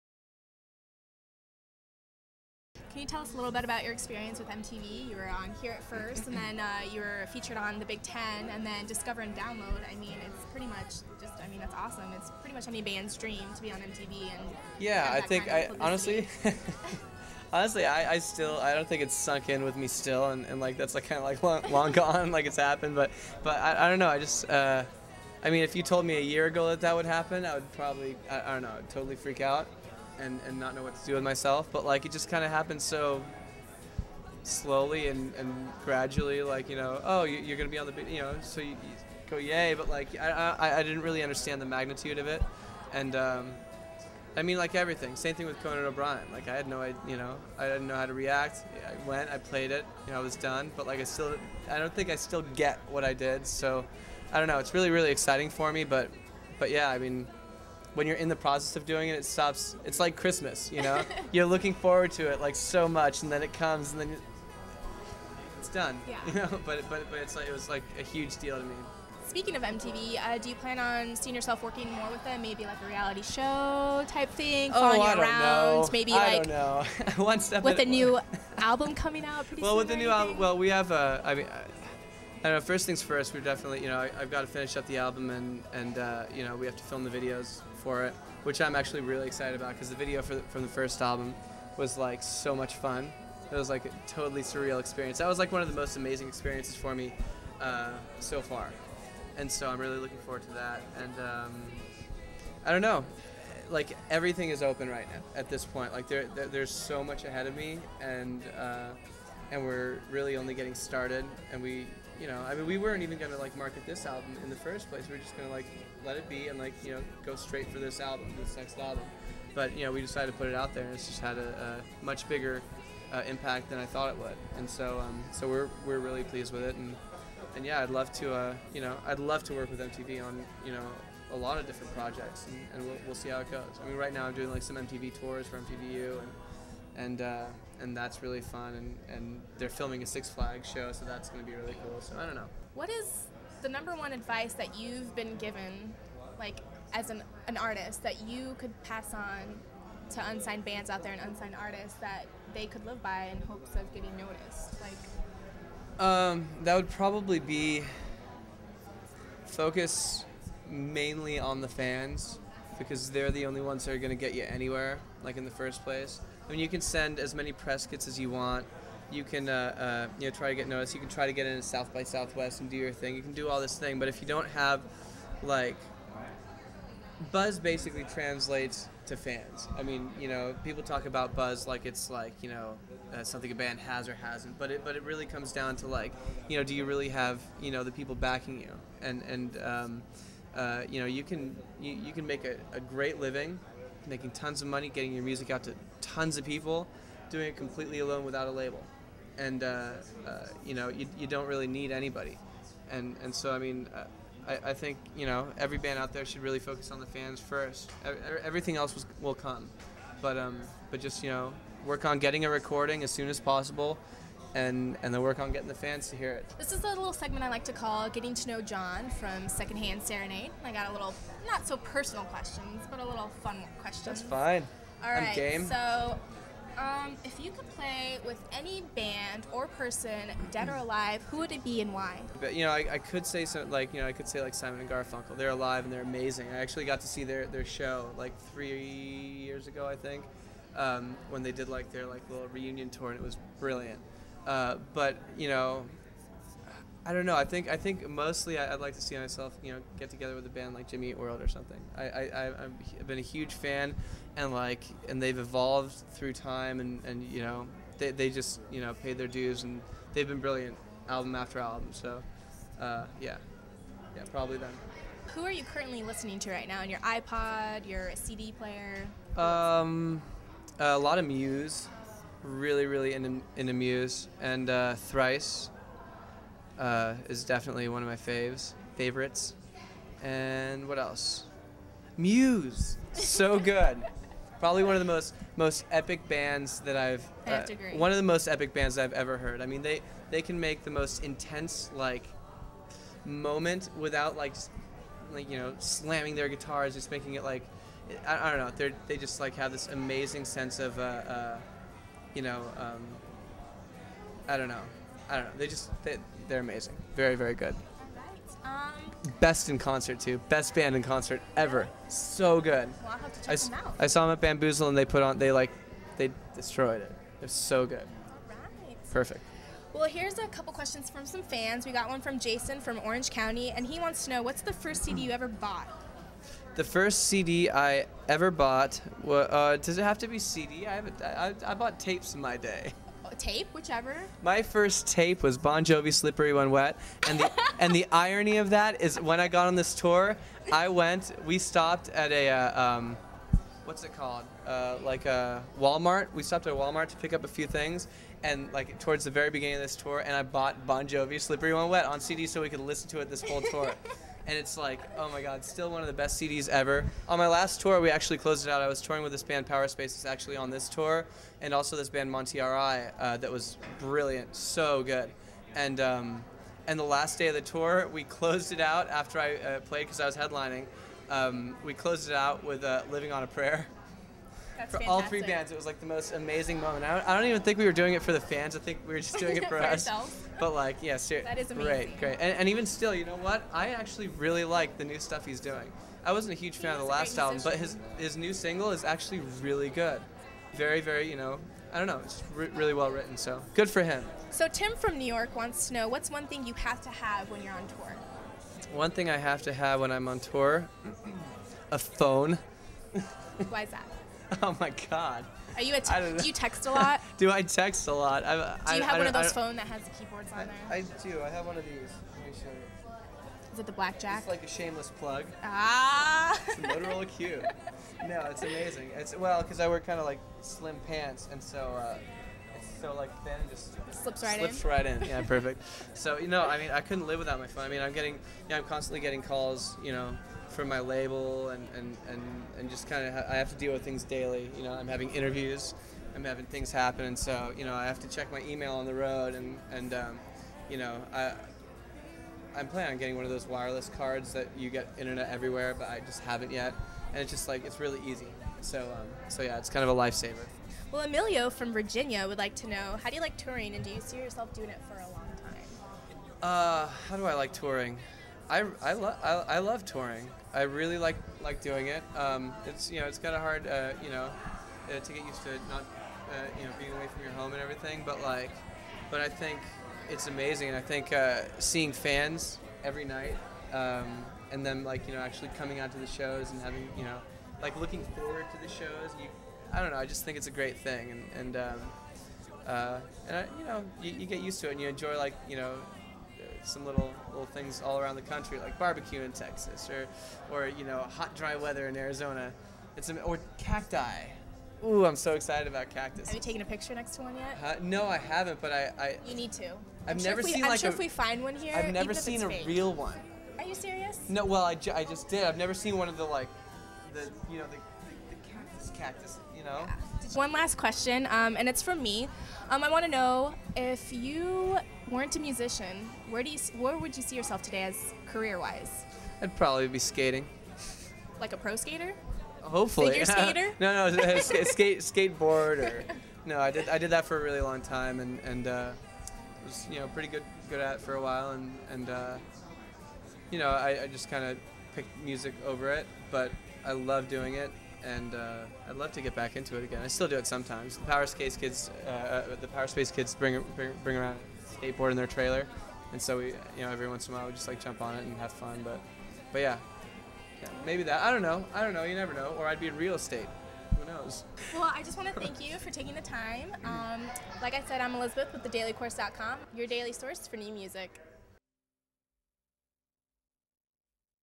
Secondhand Serenade Interview with TheDailyChorus.com Part 2
Part II of the Secondhand Serenade Interview with TheDailyChorus.com in Tempe, Arizona. For more interviews check out TheDailyChorus.com!